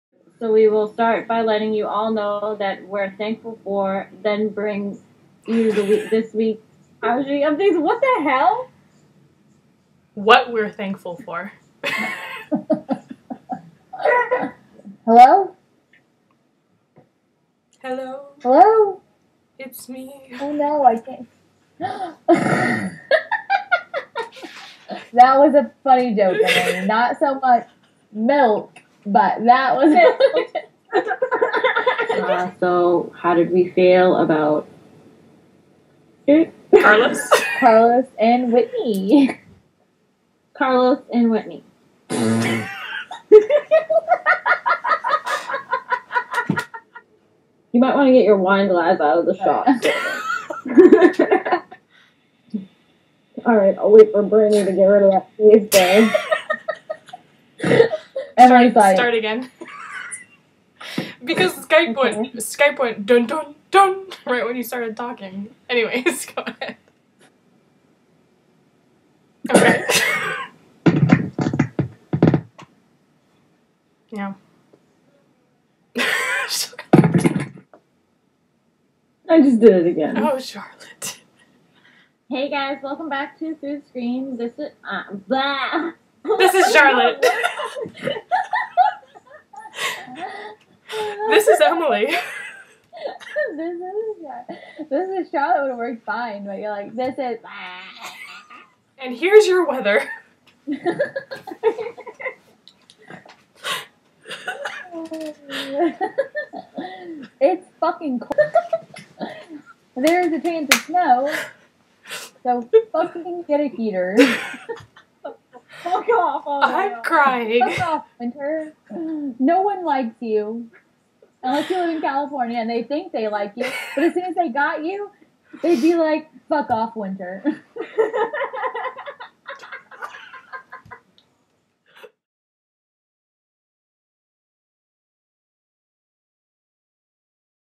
EPISODE 17 EXTRAS: Bloopers
This week's holiday episode was filled with much exhaustion and wine-induced laughter. Since you all seemed to like the last bloopers so much, please enjoy this second round of hilarious flubs. Happy Thanksgiving everyone! :)